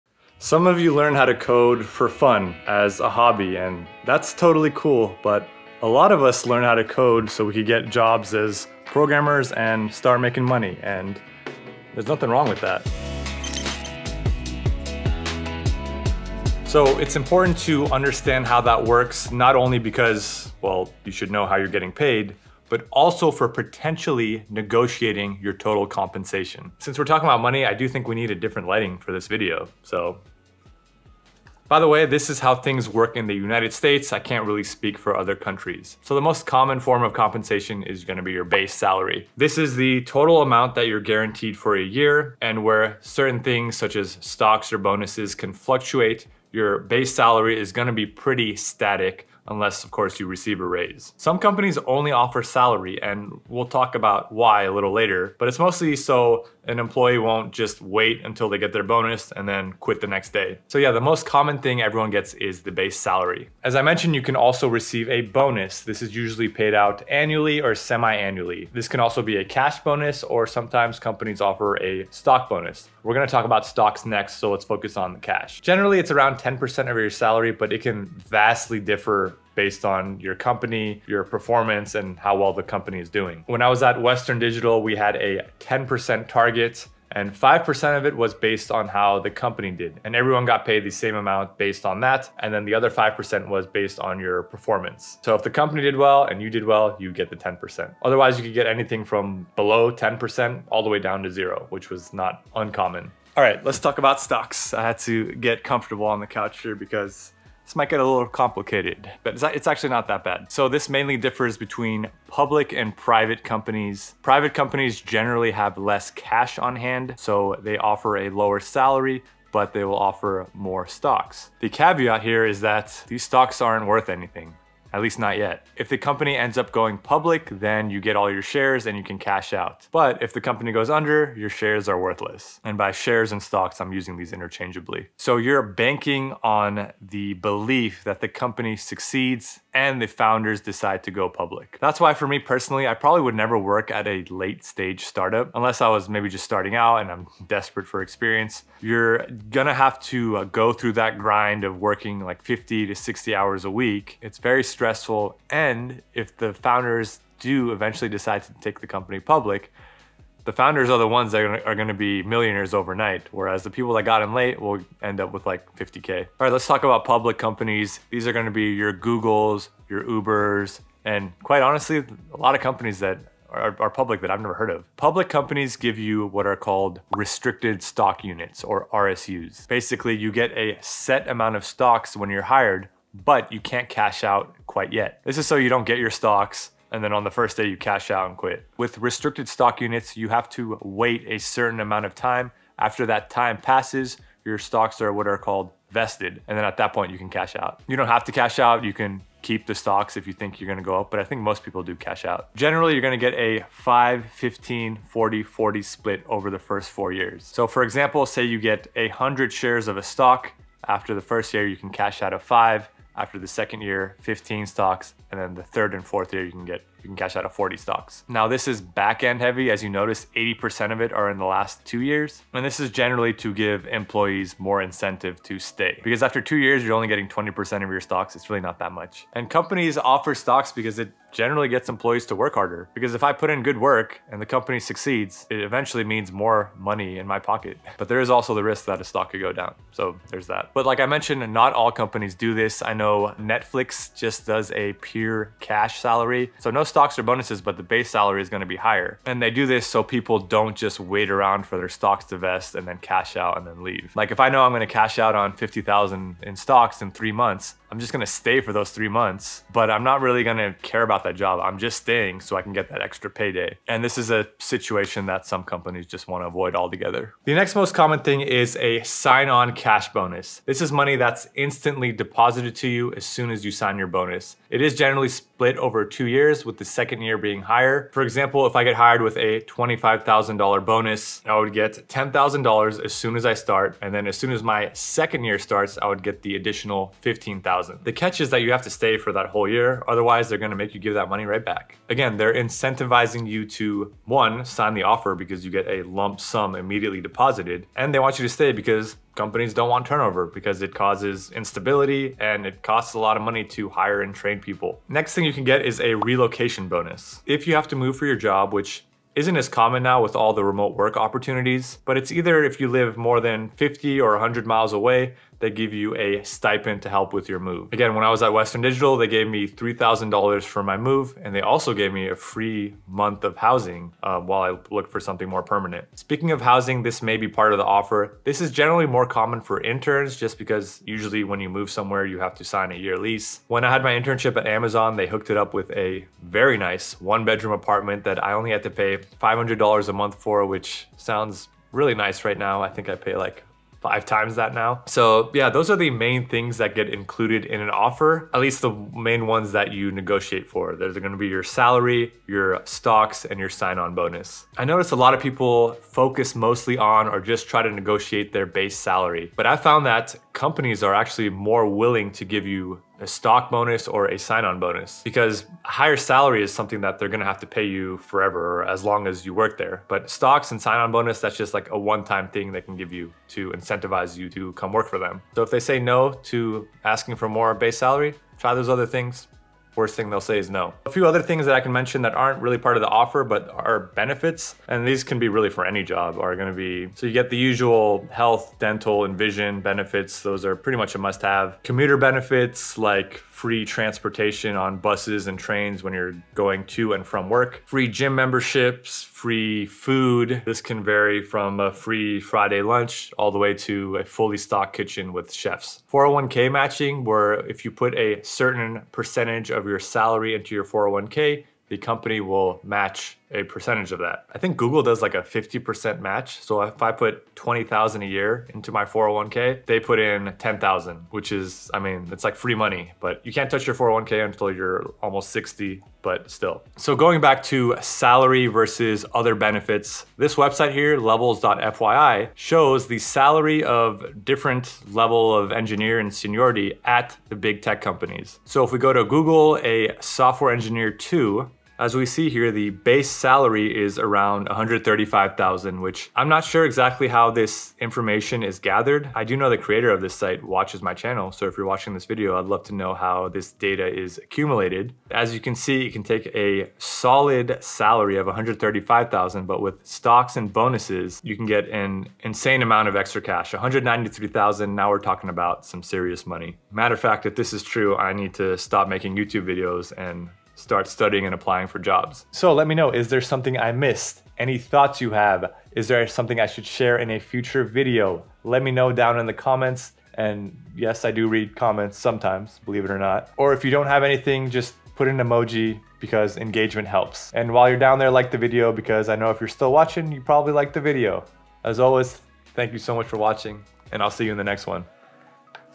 Programmer Salary Breakdown
0:00 Intro
0:36 Base Salary
1:03 Bonus
1:43 Stocks
4:27 Sign-on Bonus
5:04 Relocation Bonus
6:19 Other Benefits
7:06 Big Tech Salary & Benefits